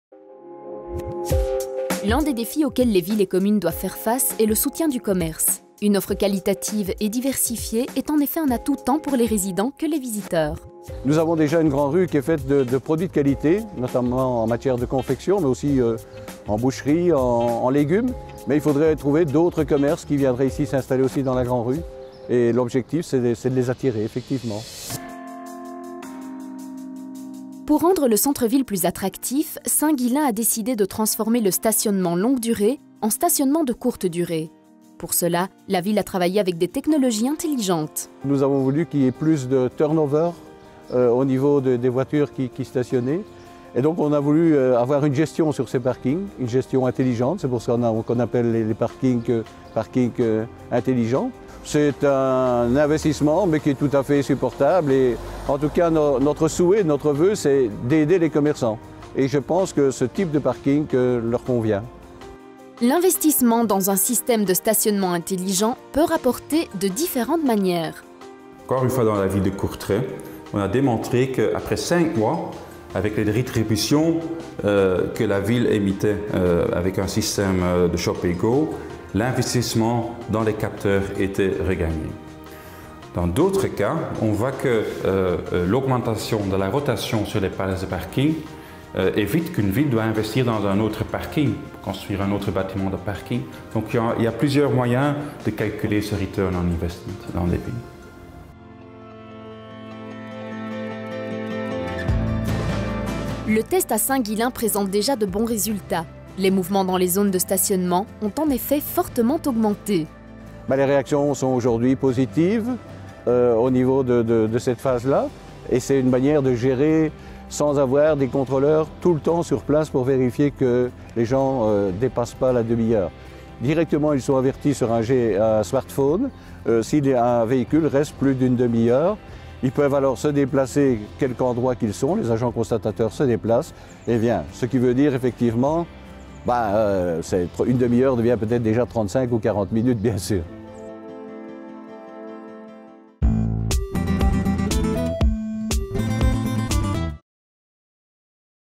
Proximus : Smart Cities : Smart parking à Saint-Ghislain
Via des systèmes de stationnement intelligent, les autorités locales peuvent proposer une offre de stationnement aisé et régulier dans leurs rues commerçantes. La ville de Saint-Ghislain a implémenté une formule innovante de stationnement de courte durée dans le cadre de sa politique de stationnement "gratuit". Cette solution a notamment été élaborée par Be-Mobile, filiale de Proximus.

Ready for tomorrow – Ook morgen mee – Prêt pour demain